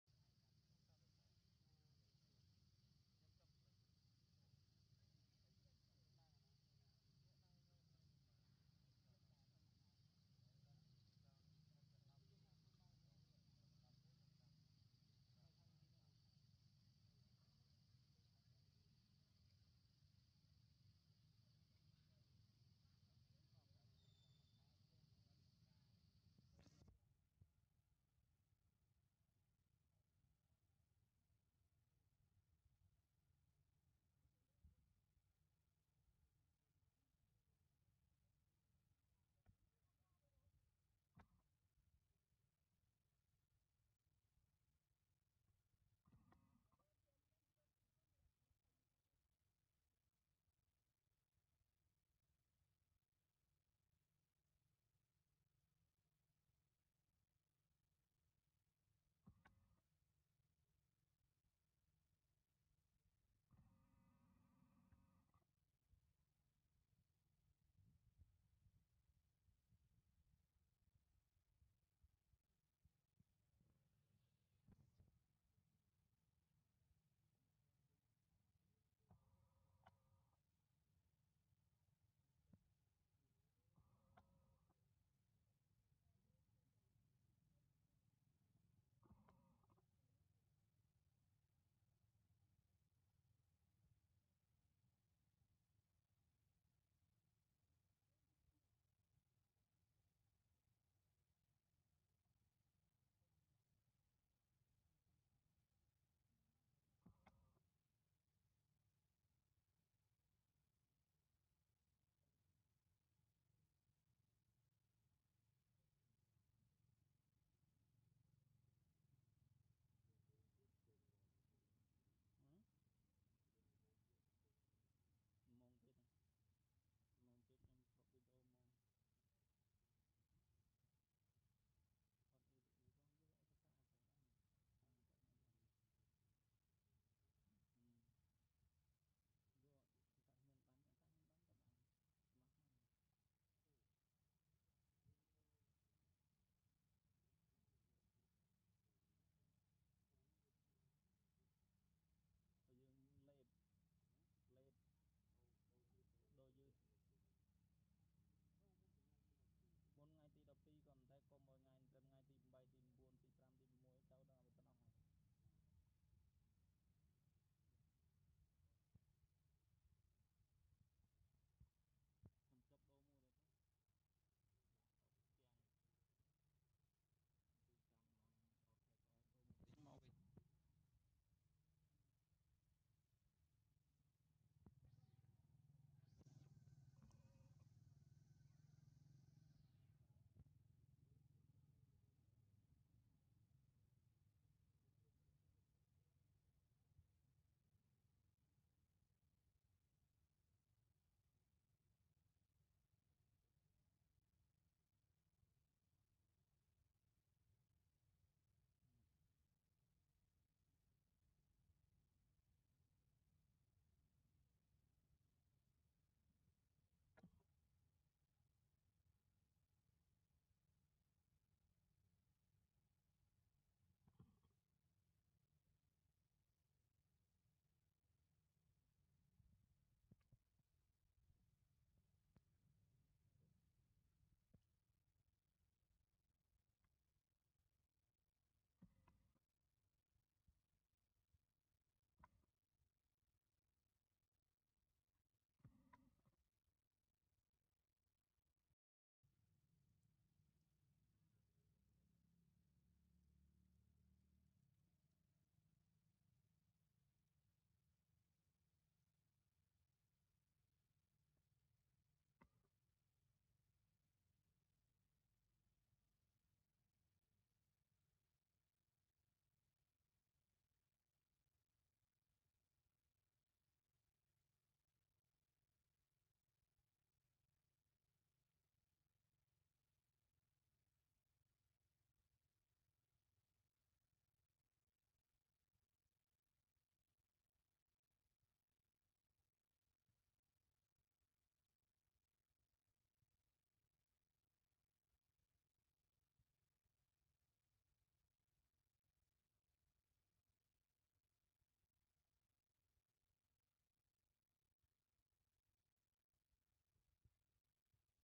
Sadness newborn baby monkey gone away, Mom so hurt because lost beloved baby
Thank you so much.Have a good days..

Thankful for watching